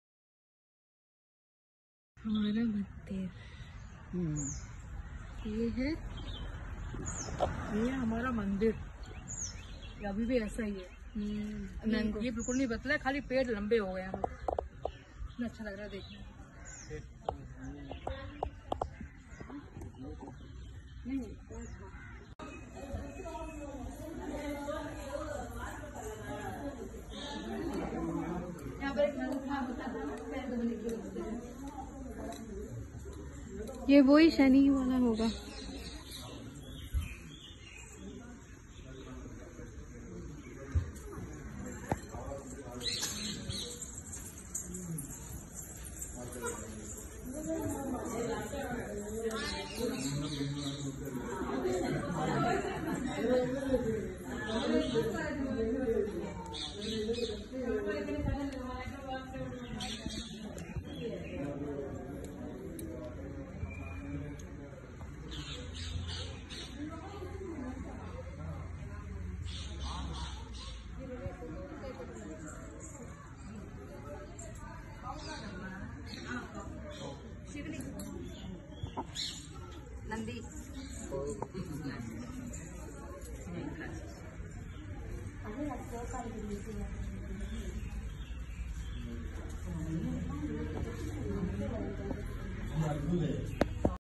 Temple Visit...
At sriharikota we visited our temple too🥰 It's same.. only the plants have turned into huge trees